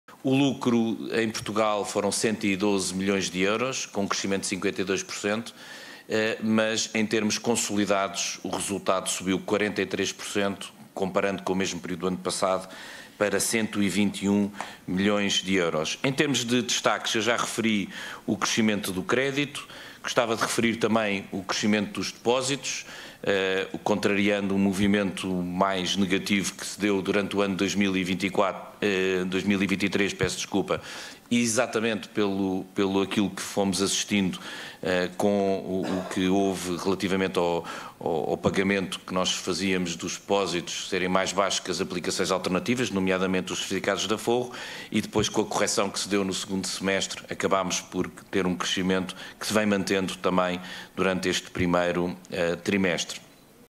Lucros do BPI sobem 43% para 121 ME no 1.º trimestre
Os lucros do BPI cresceram 43% no primeiro trimestre em termos homólogos, para 121 milhões de euros, divulgou o banco.